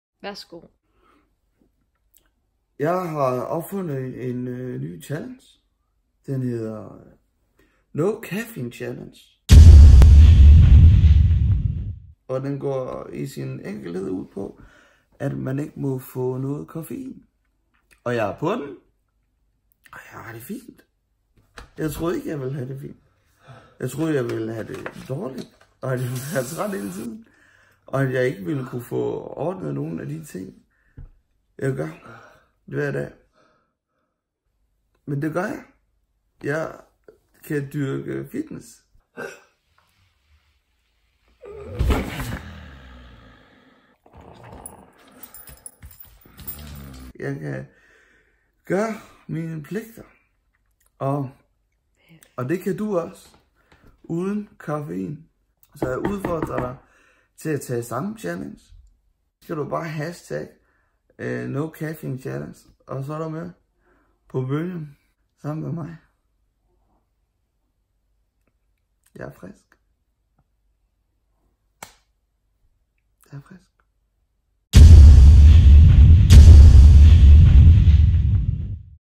NOCAFFEINECHALLENGE - “BENÆGTELSE”
Jeg var afhængig af koffein og valgte derfor at gå på “koffein-detox”. Det viste sig faktisk at være nemmere end jeg havde forestillet mig..

DEL MED EN KOFFEINAFHÆNGIG VEN!